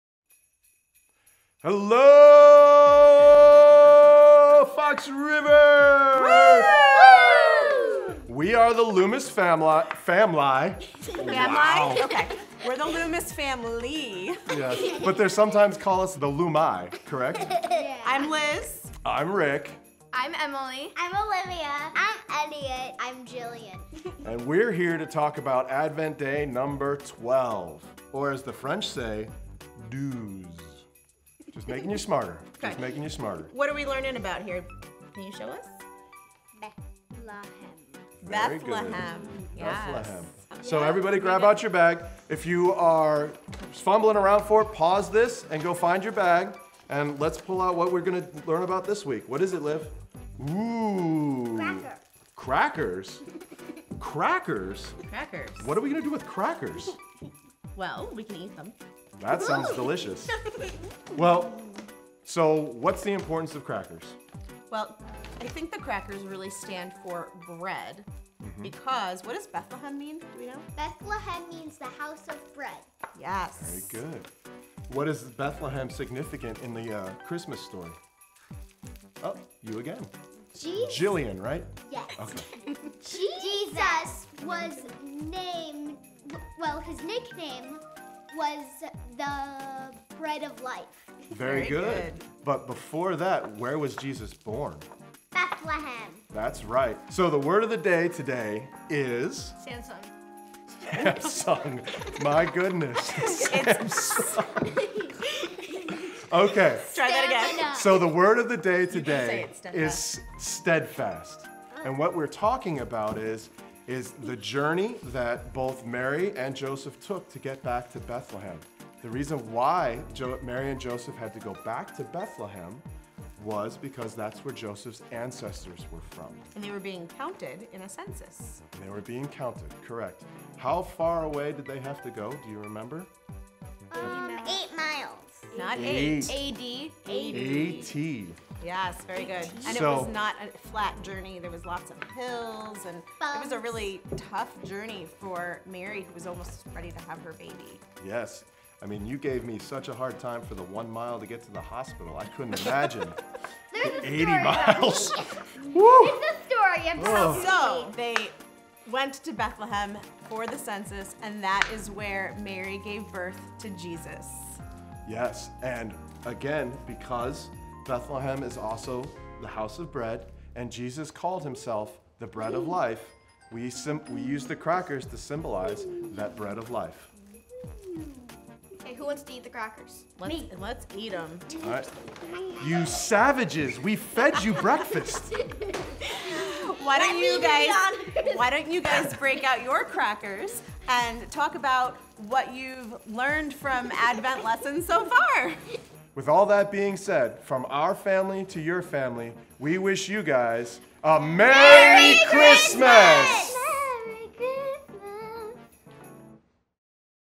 Day 12 Kids Advent | Bethlehem
Learn about Bethlehem with Rick, Liz, Emily, Olivia, Jillian, and Elliot!